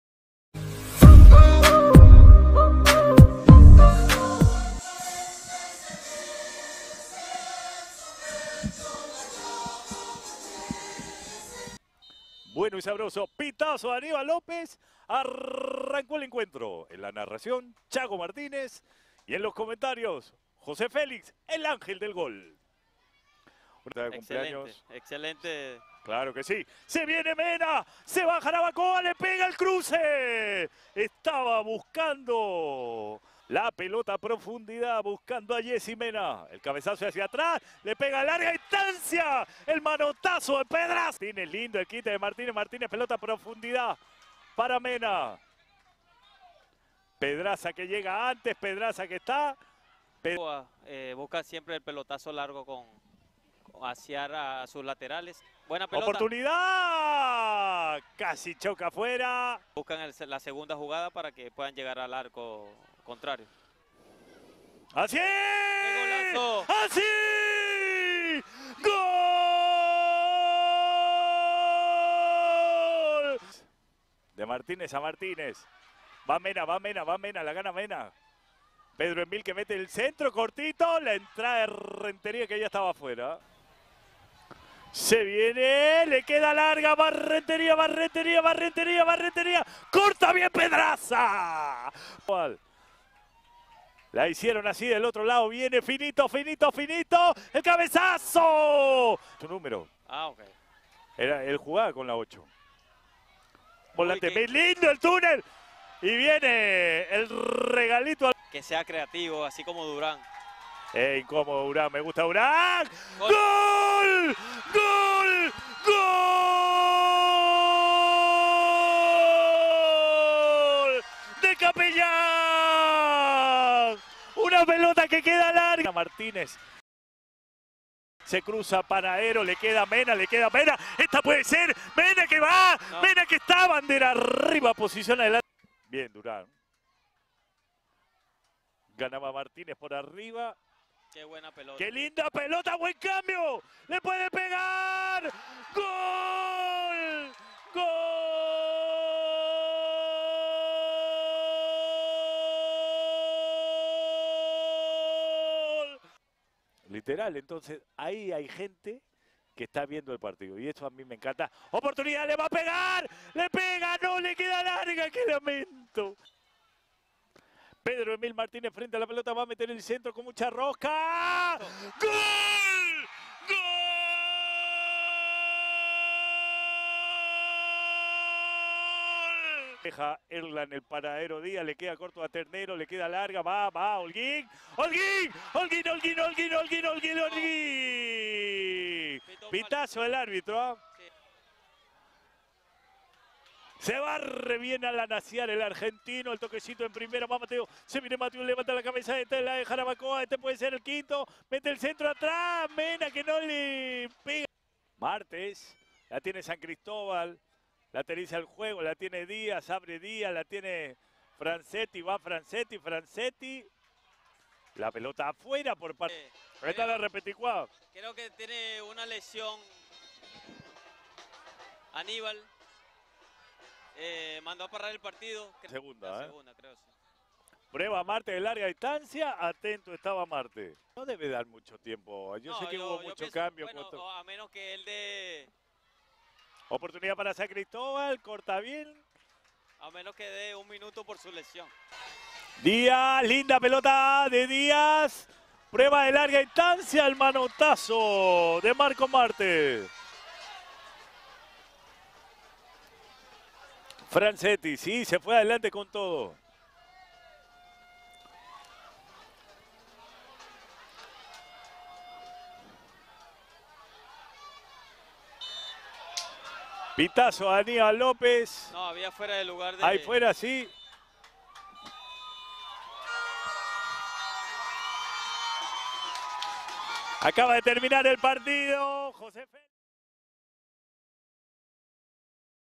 Resumen Jarabacoa FC 4-0 Atlético San Cristóbal │ Jornada 9 LDF 2022
Jarabacoa gana con autoridad